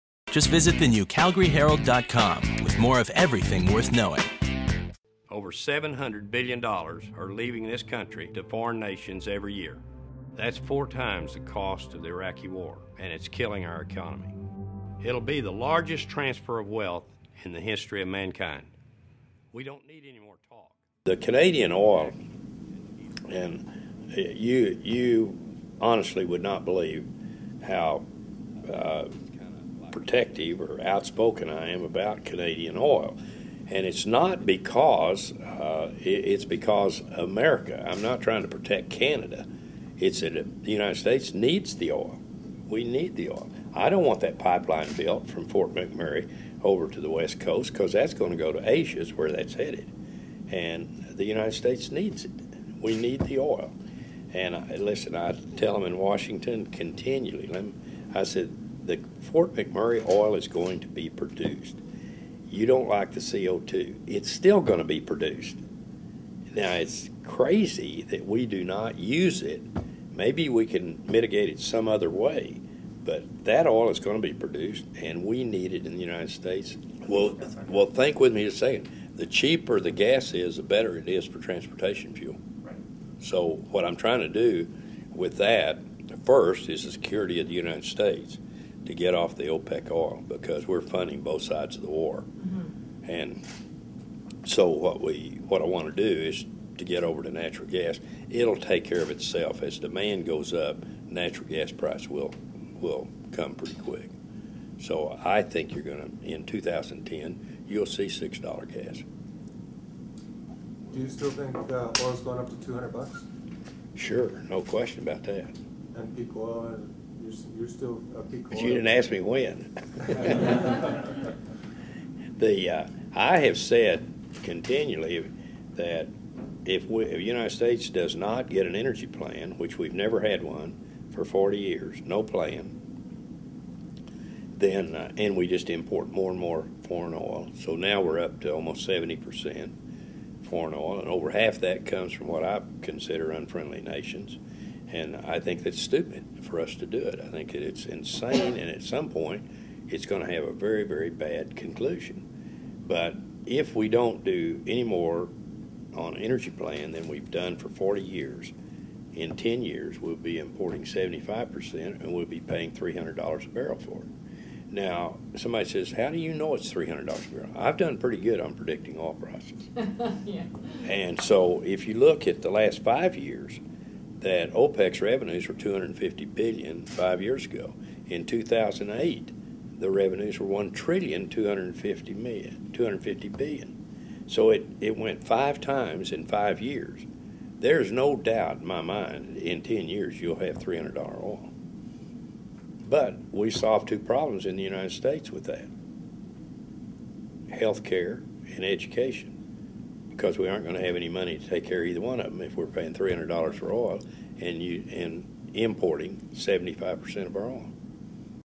A few minutes with T. Boone Pickens
Philanthropist and oilman T. Boone Pickens dropped by for a visit with the Herald editorial board this week and showed he's the kind of friend Alberta's oilsands needs in the U.S.
Although a patriotic proponent of using U.S. domestic supplies, any increase in demand for natural gas would hugely benefit Alberta, especially if Pickens' prediction proves true that the commodity will rise to $6 US by next year.  Here are a few of Pickens thoughts.